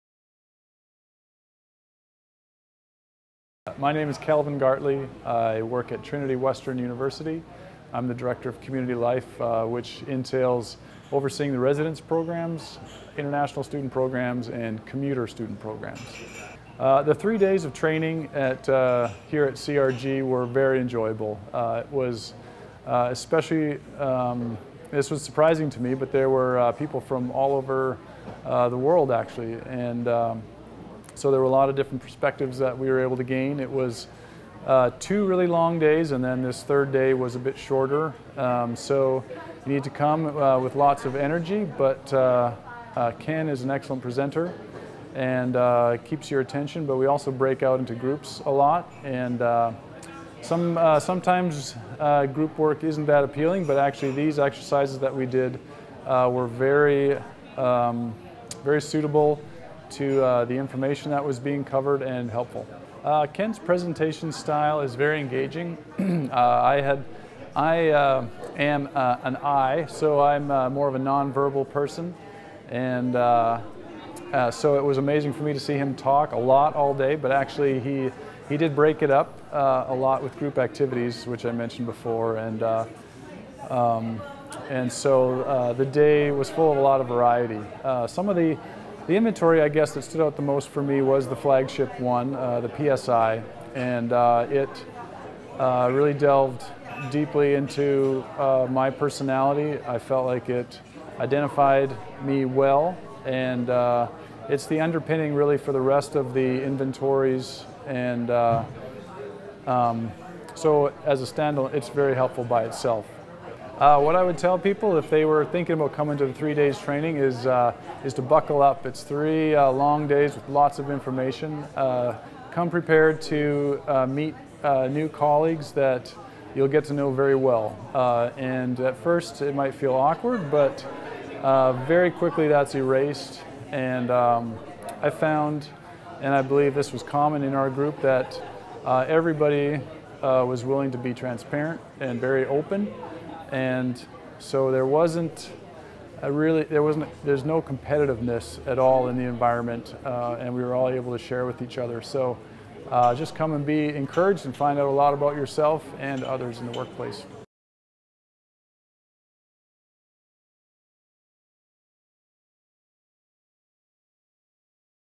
Kelvin Gartley | Director Community Life at Trinity Western University
Kelvin Gartley, Dir. Community Life, Trinity Western University, explains how CRG Assessment Systems Certification' intensive three day program helped him identify and understand his own Personal Style.